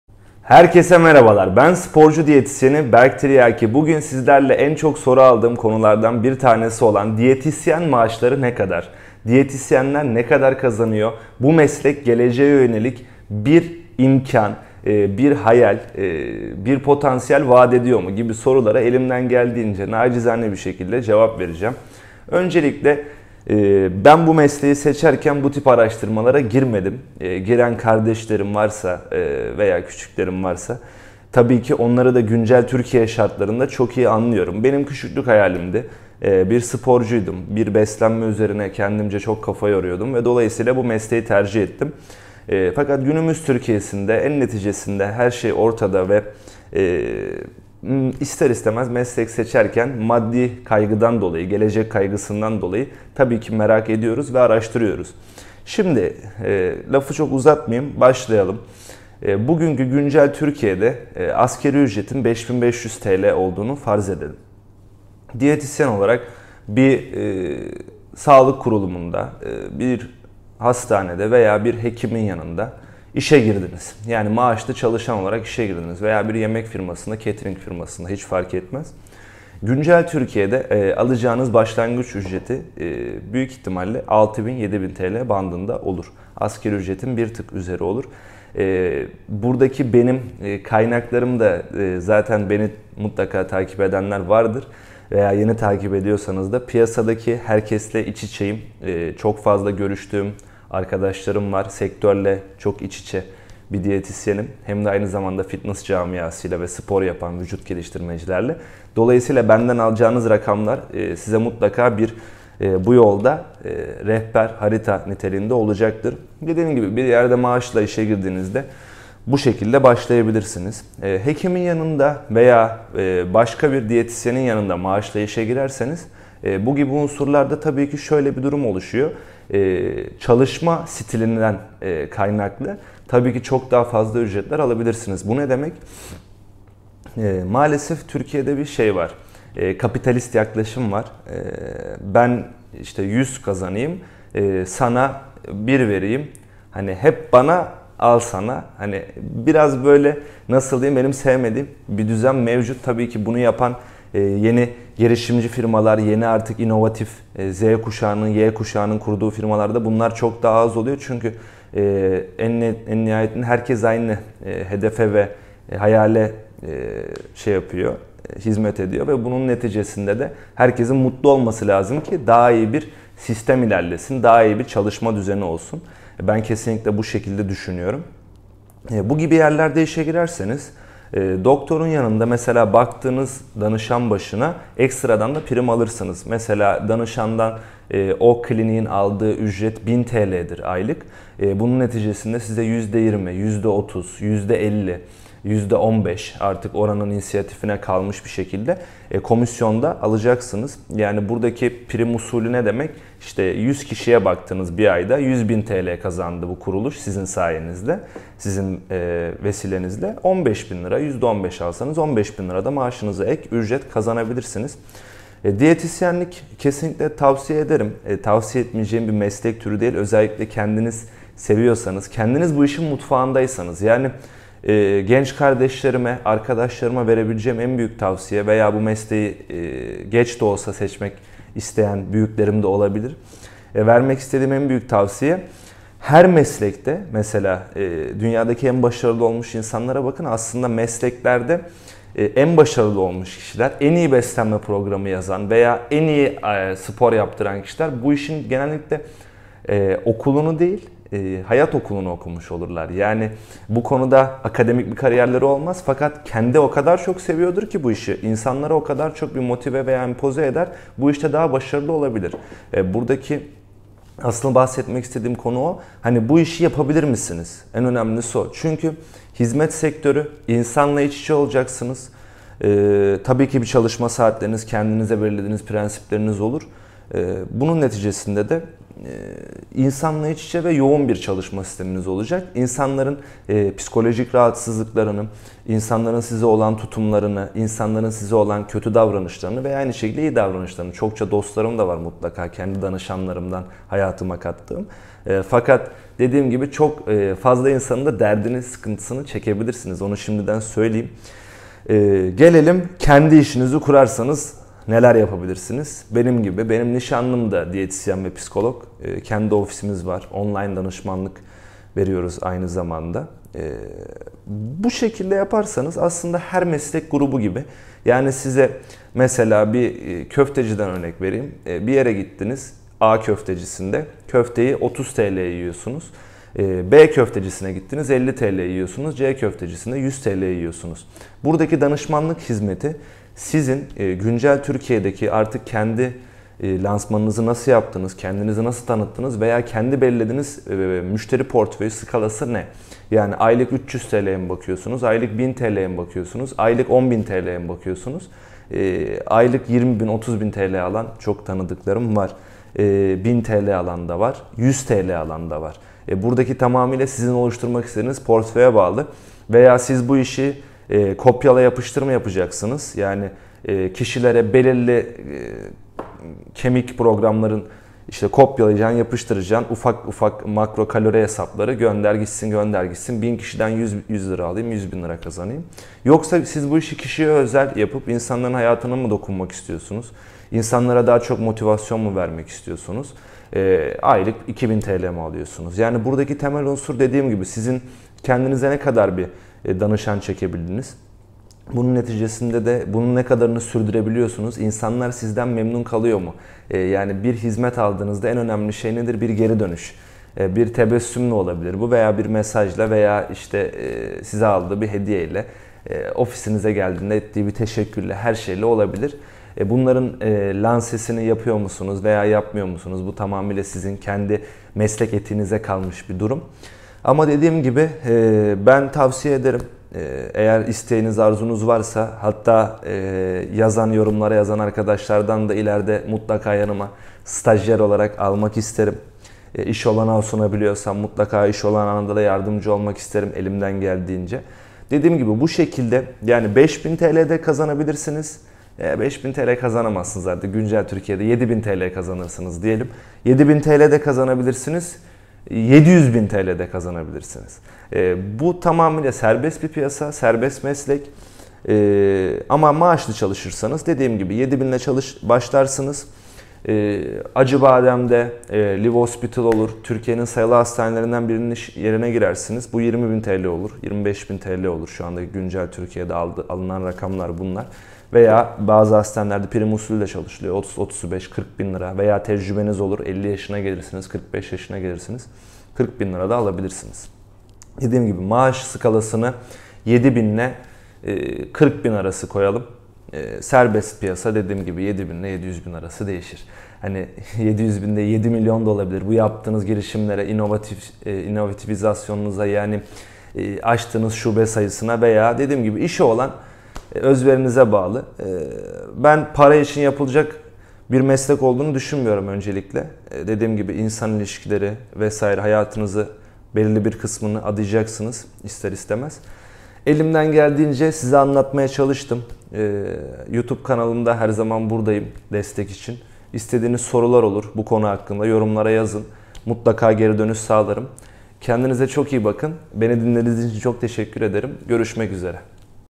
Diyetisyen Ne Kadar Kazanır ? - Diyetisyen Maaşları
Diyetisyen Ne Kadar Kazanır ? - Diyetisyen Maaşları - Diyetisyen Maaşları Ne Kadar ? Meslek Kazançları
Diyetisyen adaylarına ve meslek seçiminde olan kardeşlerime ve küçüklerime bir rehber olması için detaylı bir video çektim.
Maalesef her meslek grubunda tercih yaparken , ister istemez maddi kısmı da herkesin aklında soru işareti yaratıyor.
Diyetisyenlik veya herhangi bir meslekte hayalleriniz ve sevdiğiniz işin peşinden gitmeniz nacizane tavsiyem, para işinizi iyi yaptığınız takdirde gelecektir.
Elimden geldiğince sizler için anlattım, uzun hali youtube kanalımda mevcut, sorularınız olursa yorumlar kısmına yazın tek tek cevap vereceğim.